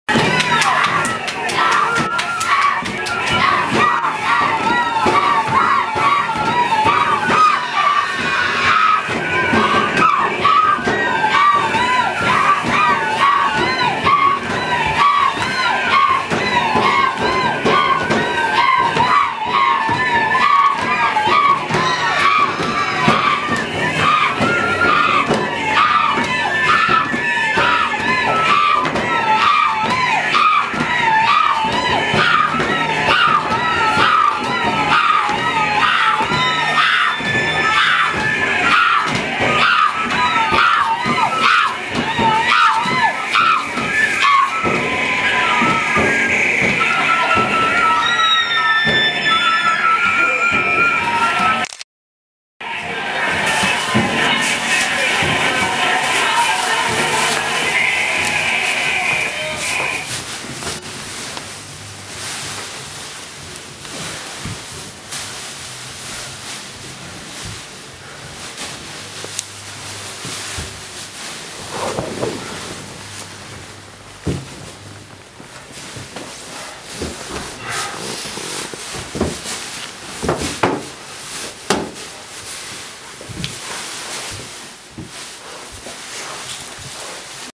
The Cabaret Voltaire at The George Tavern
Short video of Needless Alley Collective performing our The Cabaret Voltaire at The George Tavern Theatre. June 2012